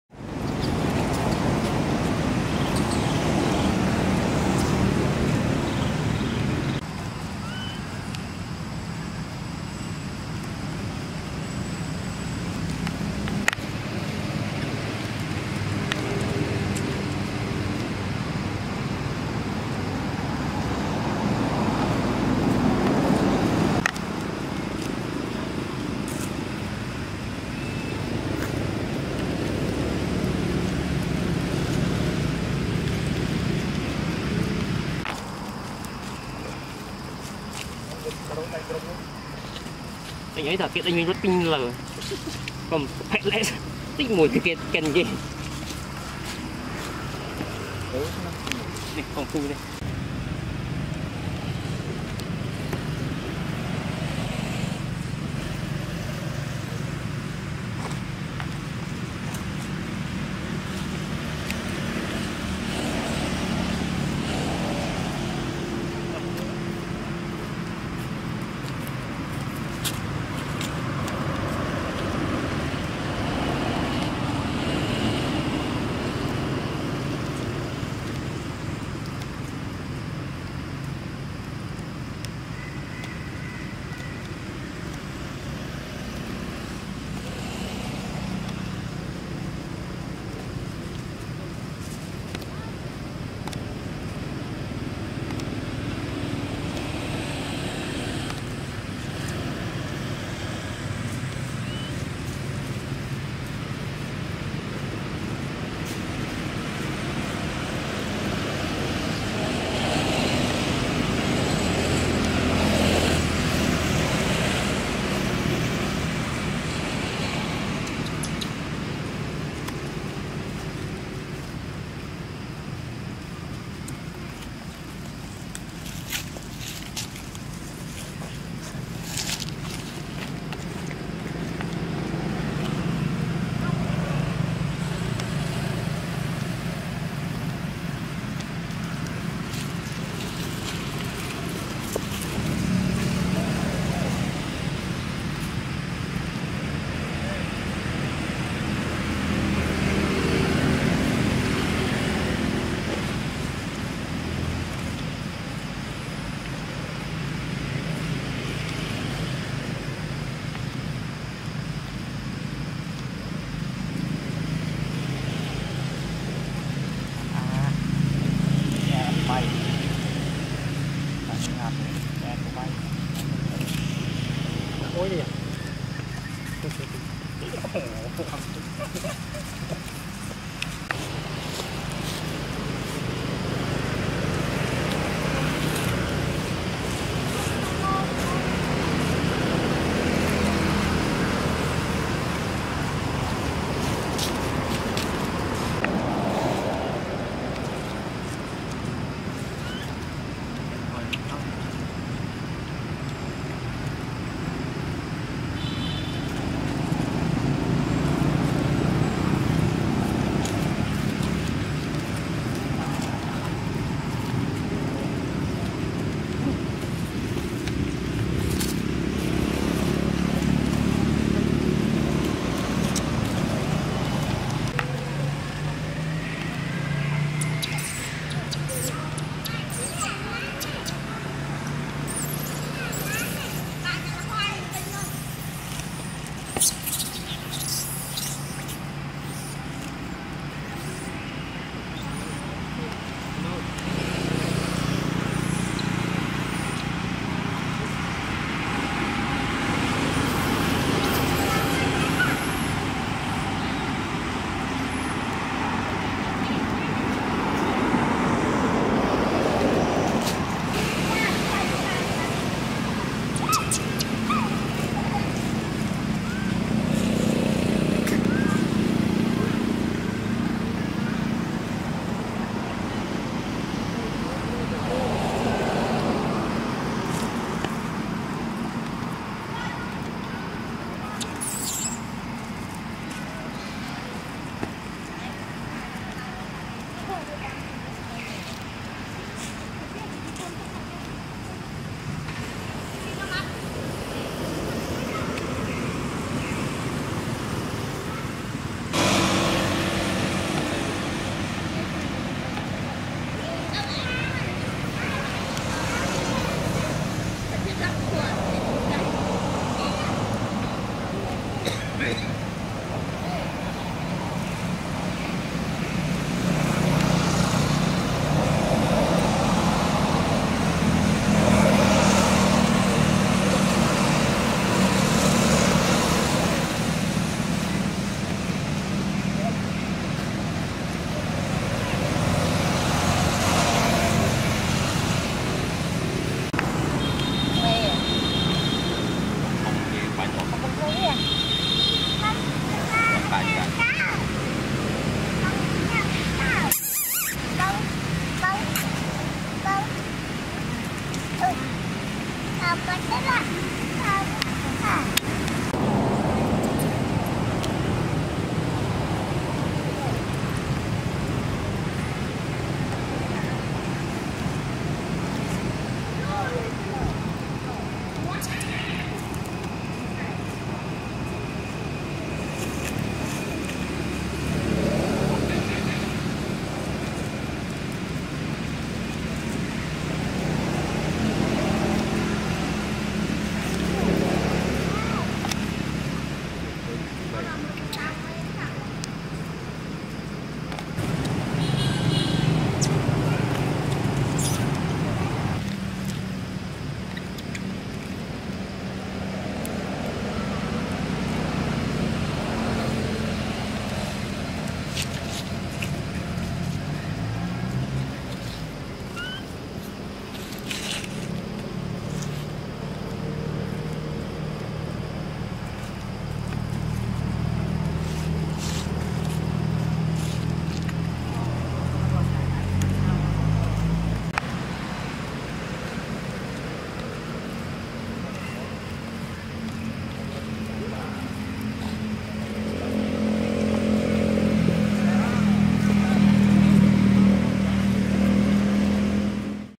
Little monkeys are living with their mother
Watch as we explore how these monkeys have been trained to live with humans and learn some of their interesting tricks. This video is sure to bring a smile to your face, and perhaps teach you a thing or two about how to better relate to others!
Monkeys are good for a lot of things, but one of the best things they are good for is teaching kids how to laugh and have fun. In this educational video, we take a look at the Little monkeys who are happy and content in their home with their long-time master.#992